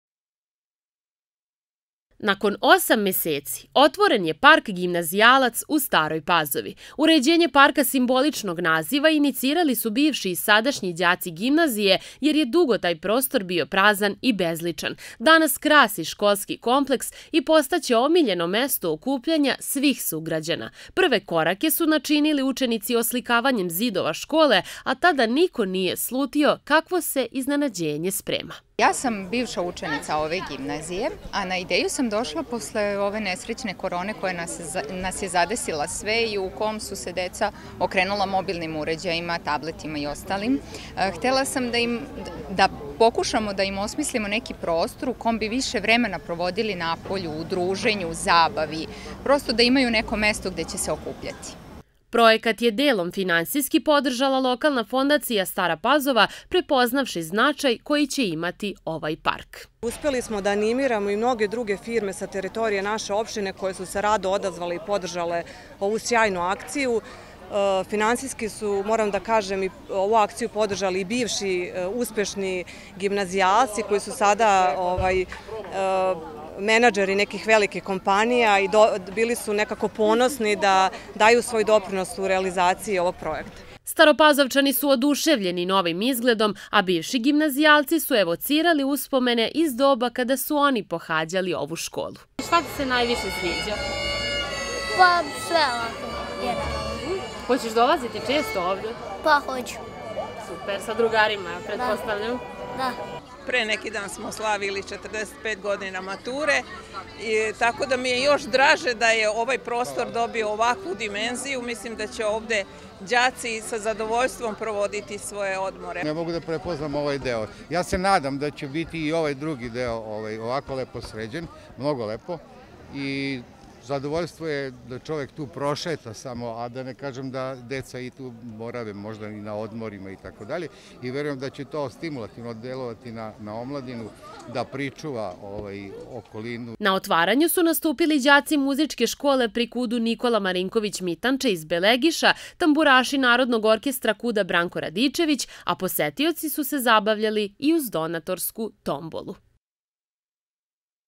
Otvoren park Gimnazijalac u Staroj Pazovi
U Staroj Pazovi je otvoren još jedan prelep, veliki park koji se simbolično zove "Gimnazijalac".  Nalazi se iza srednje ekonomske škole i gimnazije i prema rečima meštana, definitvno je ukrastio taj deo Pazove.  Projekat je podržala Lokalna fondacija uz brojne firme i pojedince a atmosferu sa samog otvaranja, pogledajte u nastavku...